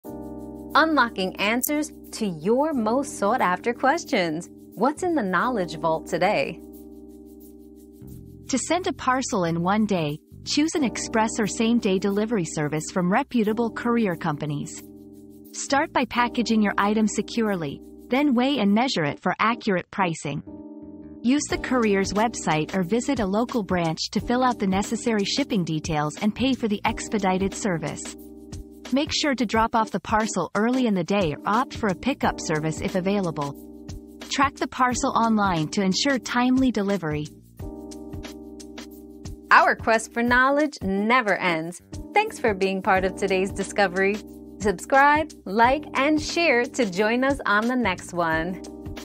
How do I send a parcel in one day?
One-Day Parcel Delivery: Quick and Easy Guide 👉 Quick Parcel Delivery 👉 Learn how to send a parcel in just one day with this simple guide. Choose an express delivery service, package your item securely, and track your parcel online for peace of mind.

Laura S. Harris (2024, April 14.) How do I send a parcel in one day?
    🌐 AskAbout.video